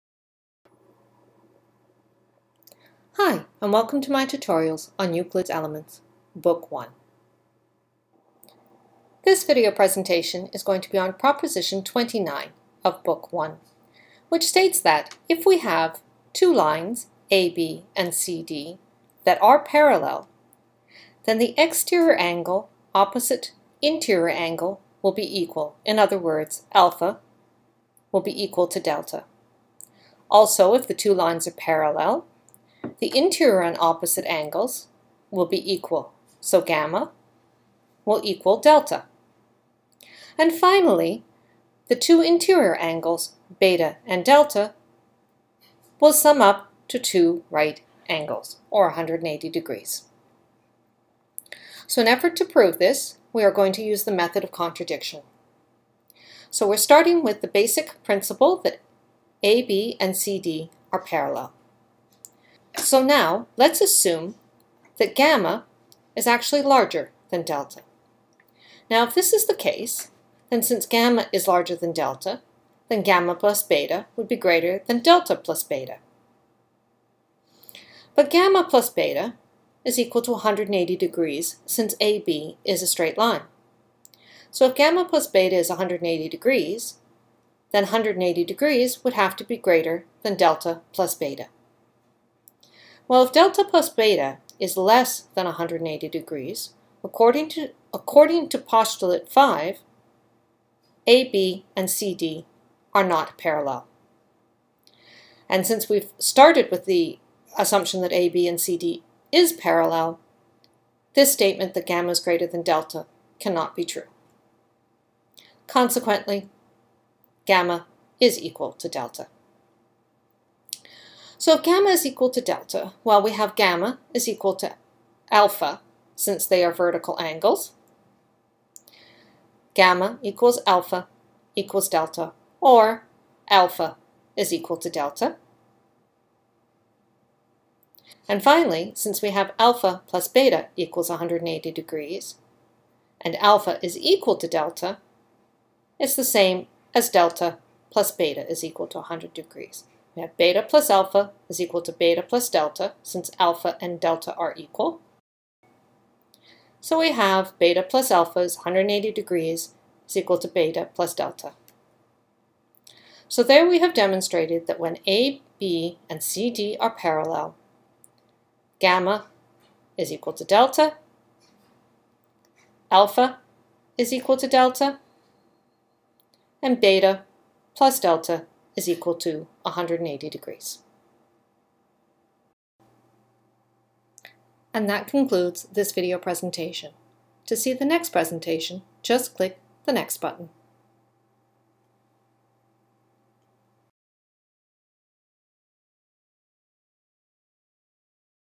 Euclid's Elements Book 1 - Proposition 29
If a line falls on two parallel lines, then the interior and opposite external angles are equal, the alternate angles are equal, and the sum of the interior angles is 180 degrees.

First use of postulate 5